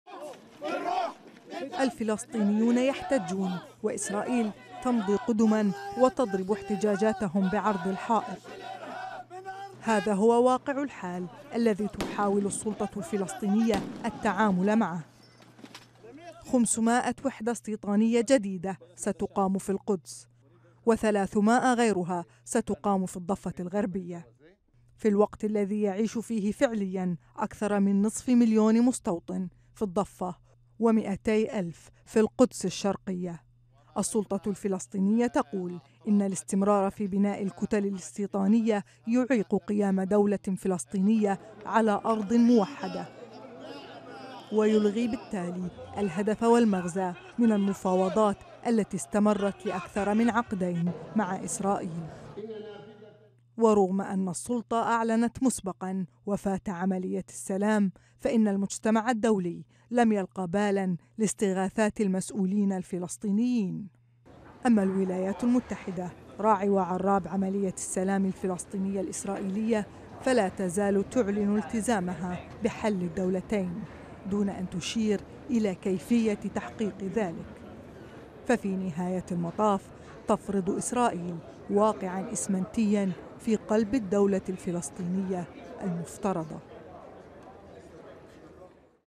الاستيطان الإسرائيلي.. توسع متواصل
لا تتوقف إسرائيل عن إجراءاتها الهادفة إلى التوسع الاستيطاني، وفي وقت تتحدث به أنباء عن لقاءات سرية تتم بين فلسطينيين وإسرائيليين لبحث وقف هذا الزحف، تفاجئ الحكومة الإسرائيلية الجميع ببناء وحدات استيطانية جديدة.